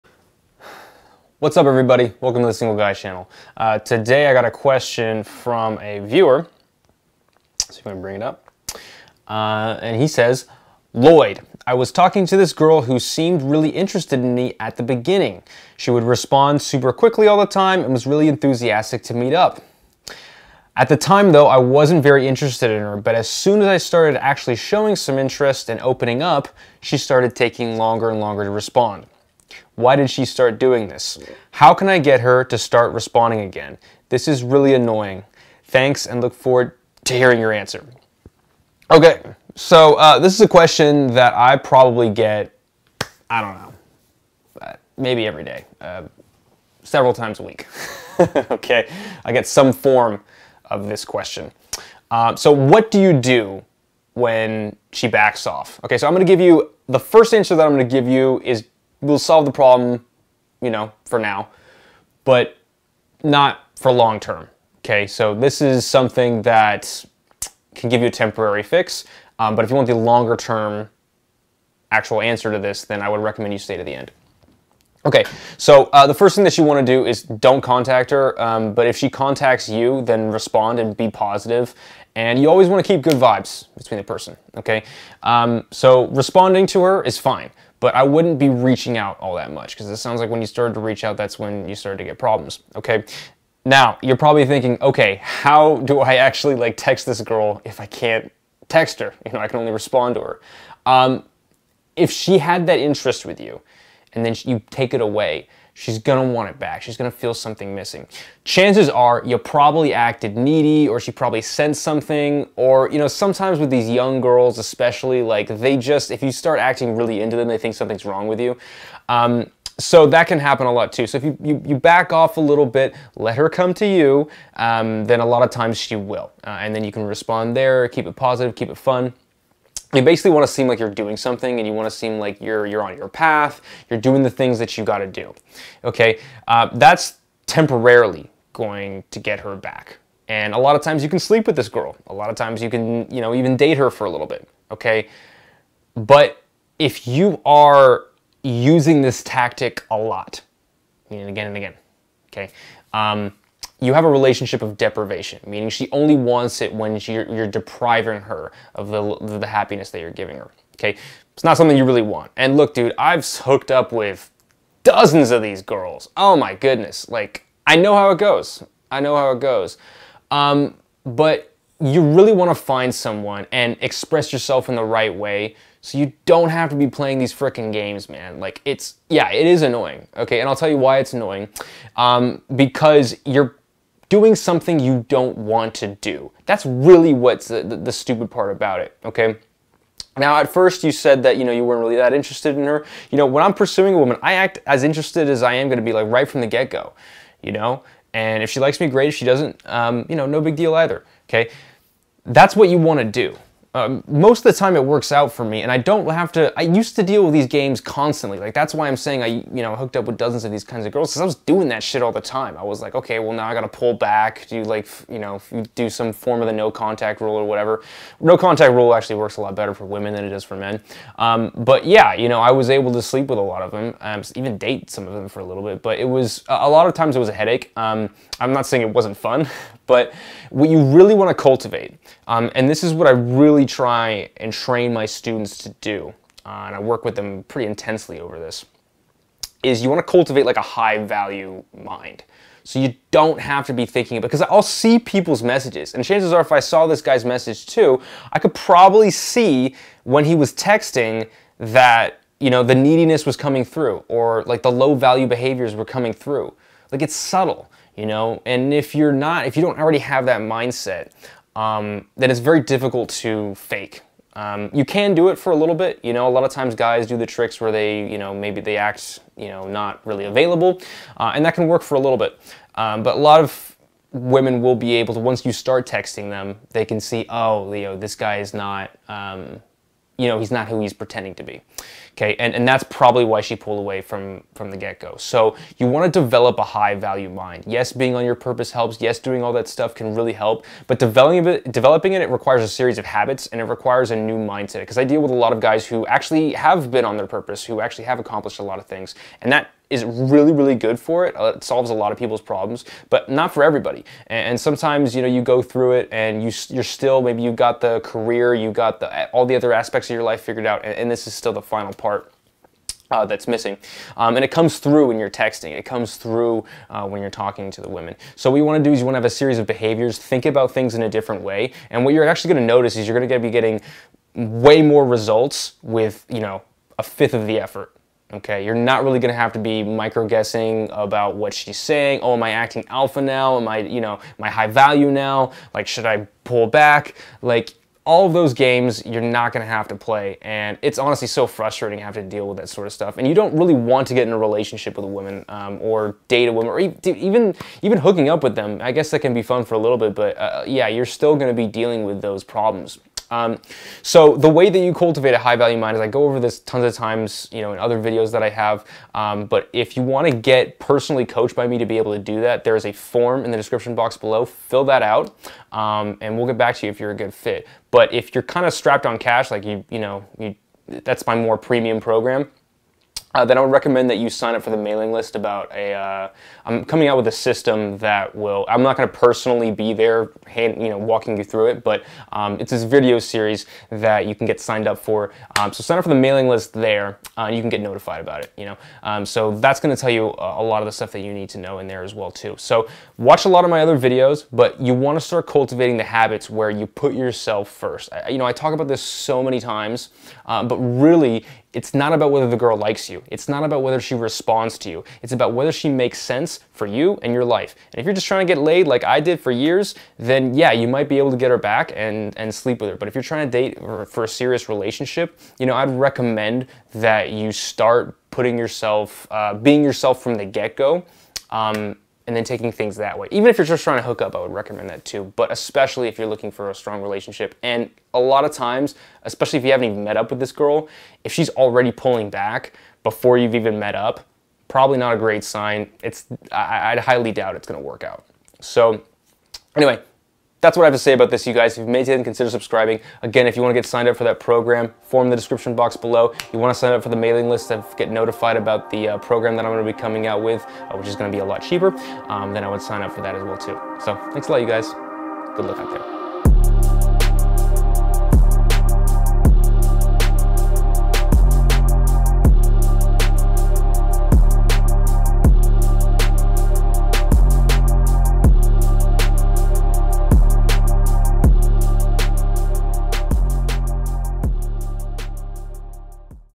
Do this when she pulls away
I get this question about almost every other day and I can give you a quick fix or the more longer term solution. In this video I give both. Start by backing off and letting her come to you.

  / a.single.guy